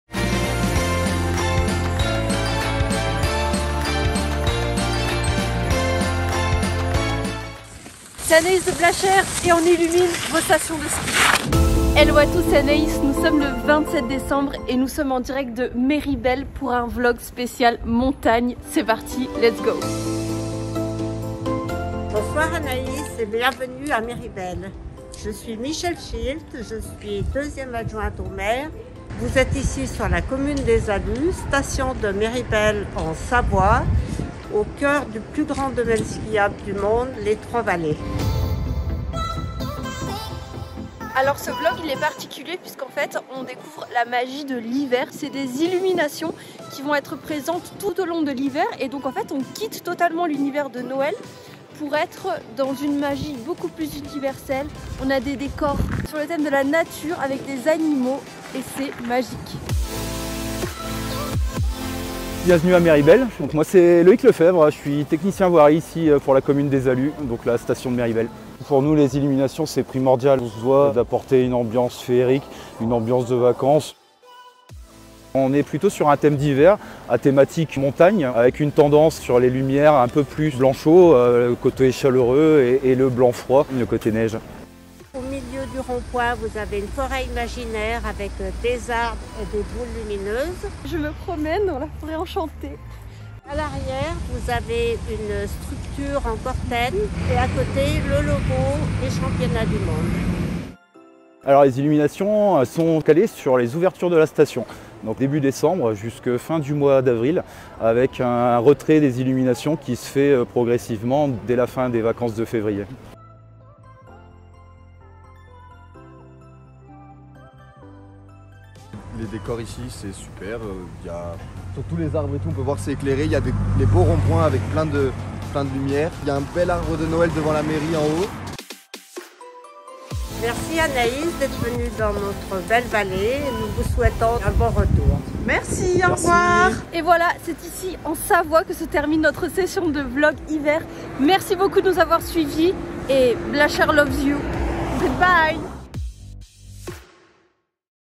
VLOG #13 LA MAGIE DE L'HIVER A MERIBEL❄️
Les illuminations en Station c'est tout l'hiver ! Et oui, on vous embarque dans la Magie de l'hiver ❄️